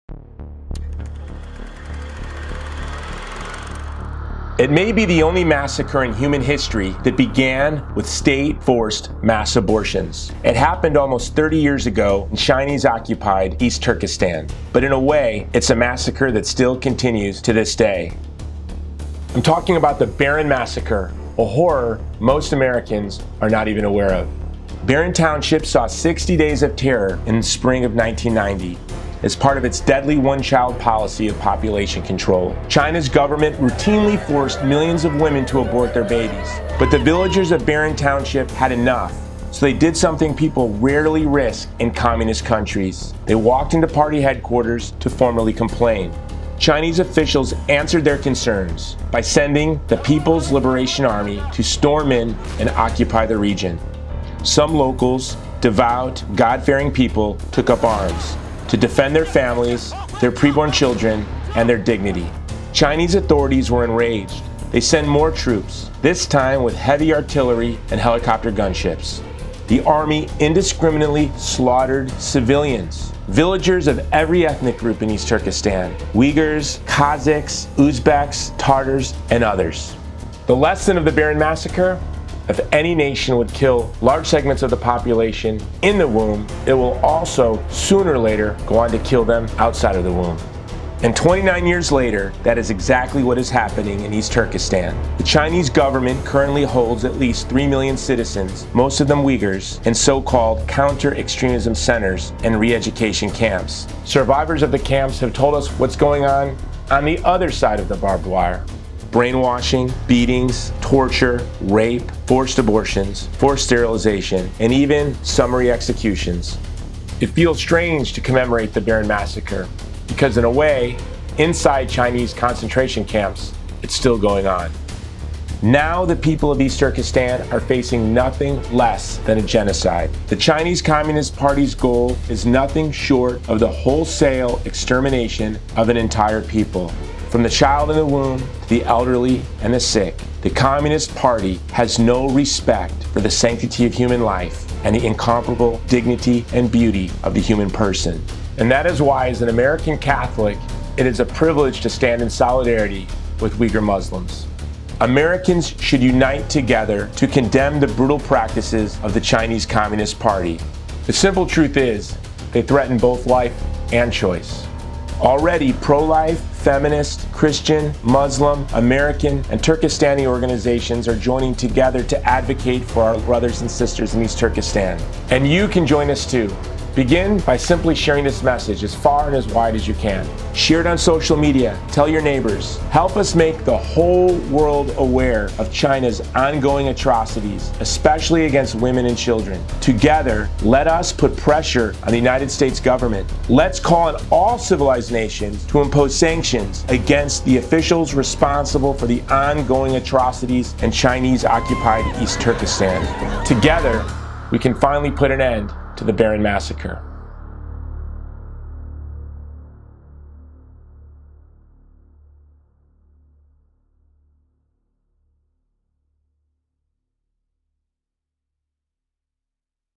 China and the Uyghur: From Forced Abortion to Genocide
The Baren Massacre may be the only massacre in human history that began with State-forced mass abortions. And in a way, it’s a massacre that still continues to this day.